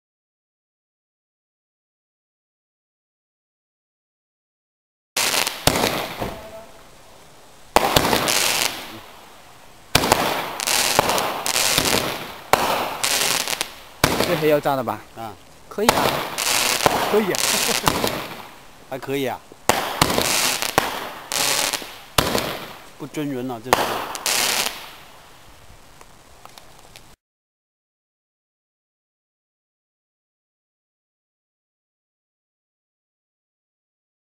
Fire & Ice.wmv
Hands Cake- This video begins at shot # 6- will add complete video when available- You should still get a general idea of the effects of this piece.